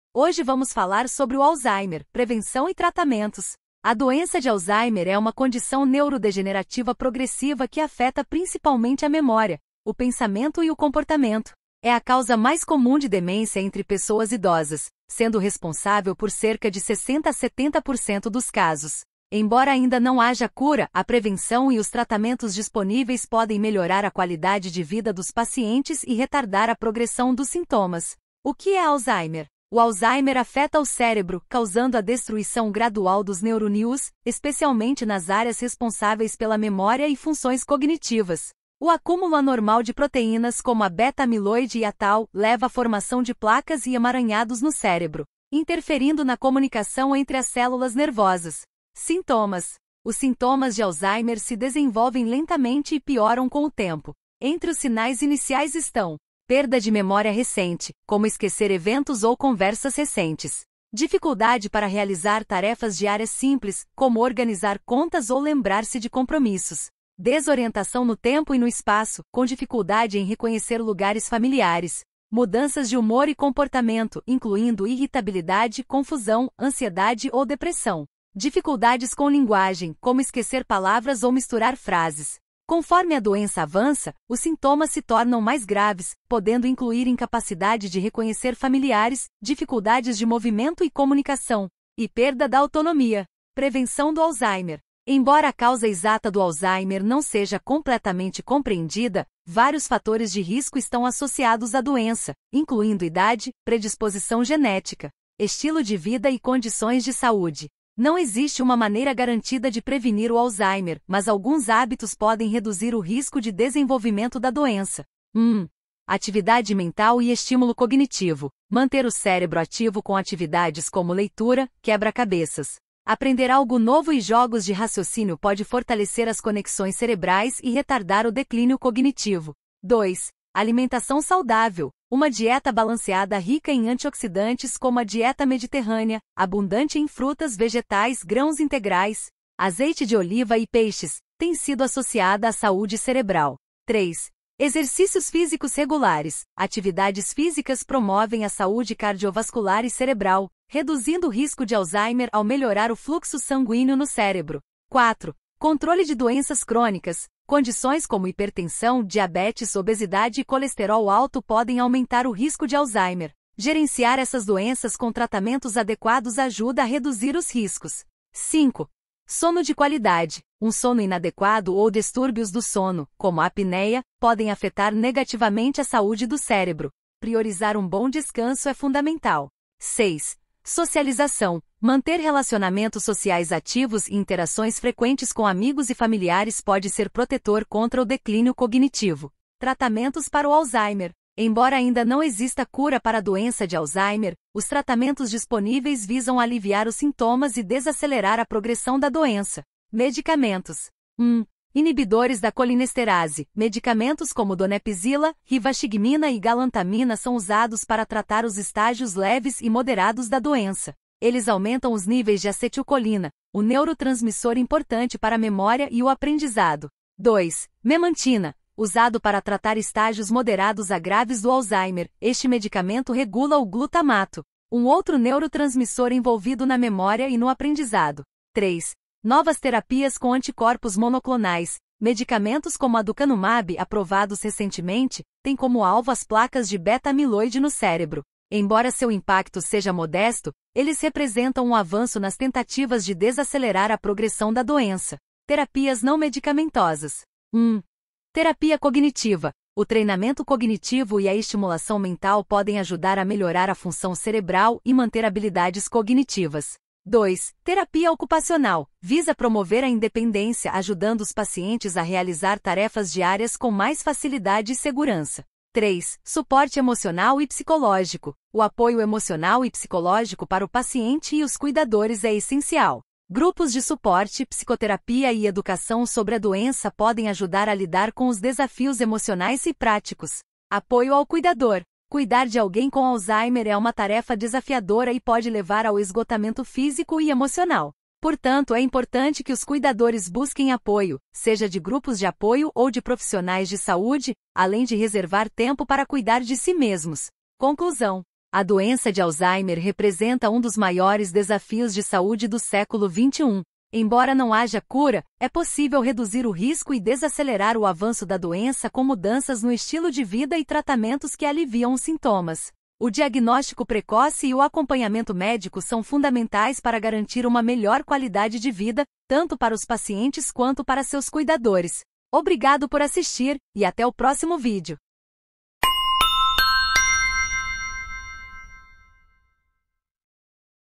Alzheimer - Prevenção, Tratamentos e o que fazer?
Neste vídeo vamos tratar sobre Alzheimer - Prevenção, Tratamentos e o que fazer?

Receba dicas e informações importantes sobre alimentação, saúde e qualidade de vida.